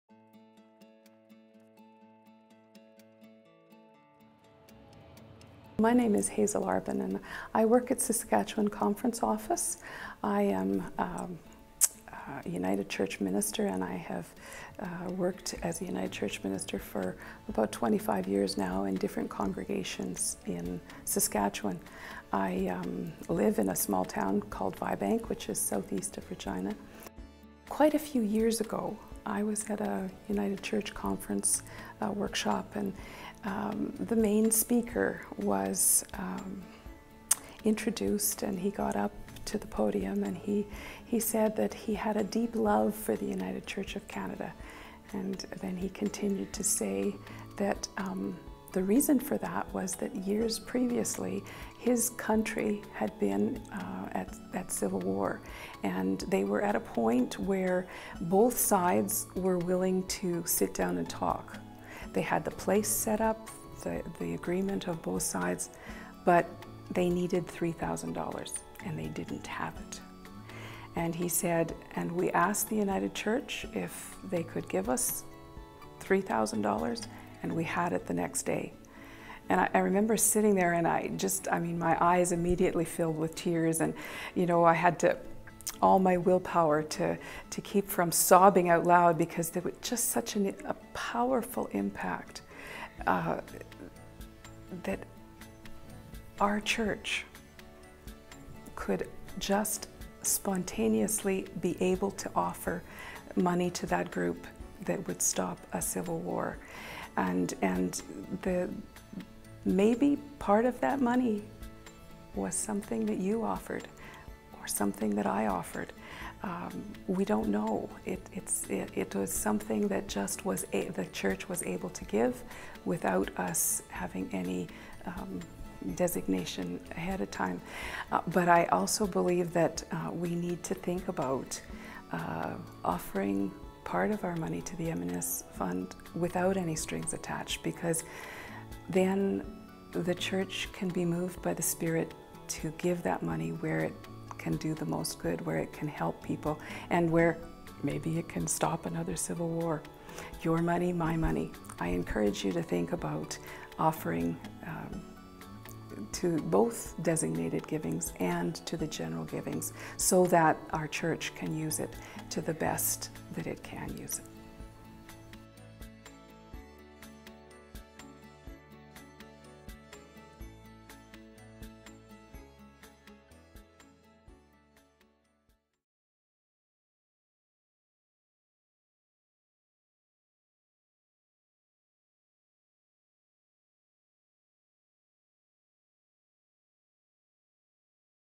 The Power of Not Knowing
Hazel Arbon of Saskatchewan delights in knowing that her donations to the Mission and Service Fund can be used in ways she could never imagine, like stopping a civil war.